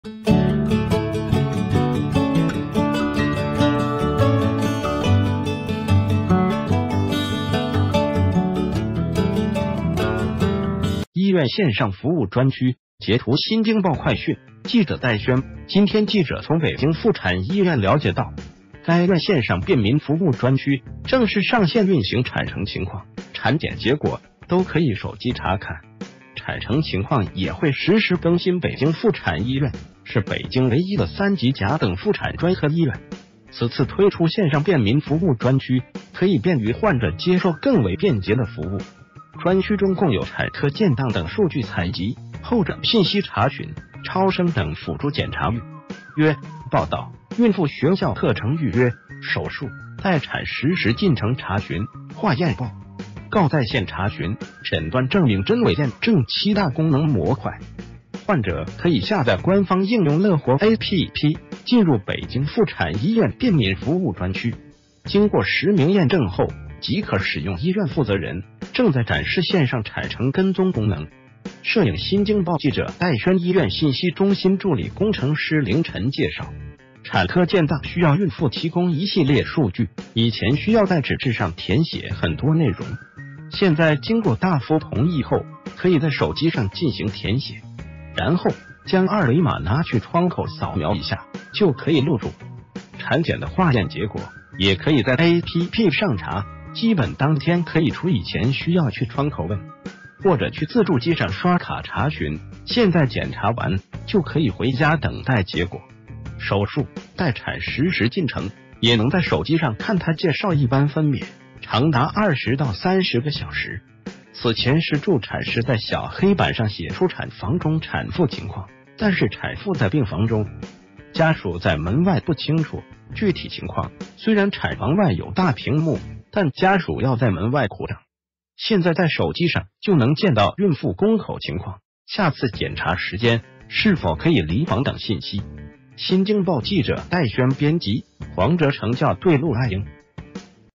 北京婦產醫院推新服務：手機可查產程信息、產檢結果
谢谢！

    醫院線上服務專區截圖。
    新京報快訊(記者 戴軒)今天，記者從北京婦產醫院了解到，該院線上便民服務專區正式上線運行，產程情況、產檢結果都可以手機查看，產程情況也會實時更新。
    北京婦產醫院是北京唯一的三級甲等婦產專科醫院。此次推出線上便民服務專區，可以便于患者接受更為便捷的服務。
    專區中共有“產科建檔等數據采集”、“候診信息查詢”、“超聲等輔助檢查預約、報到”、“孕婦學校課程預約”、“手術、待產實時進程查詢”、“化驗報告在線查詢”、“診斷證明真偽驗證”七大功能模塊。患者可以下載官方應用“樂活APP”，進入北京婦產醫院便民服務專區，經過實名驗證后即可使用。
    醫院負責人正在展示線上產程跟蹤功能。 攝影/新京報記者 戴軒
    醫院信息中心助理工程師凌晨介紹，產科建檔需要孕婦提供一系列數據，以前需要在紙質上填寫很多內容，現在經過大夫同意后，可以在手機上進行填寫，然后將二維碼拿去窗口掃描一下就可以錄入。產檢的化驗結果也可以在app上查，基本當天可以出，以前需要去窗口問，或者去自助機上刷卡查詢，現在檢查完就可以回家等待結果。
    手術、待產實時進程也能在手機上看。他介紹，一般分娩長達20到30個小時，此前是助產士在小黑板上寫出產房中產婦情況，但是產婦在病房中，家屬在門外，不清楚具體情況，雖然產房外有大屏幕，但家屬要在門外苦等。現在在手機上就能見到孕婦宮口情況、下次檢查時間、是否可以離房等信息。
    新京報記者 戴軒 編輯 黃哲程 校對 陸愛英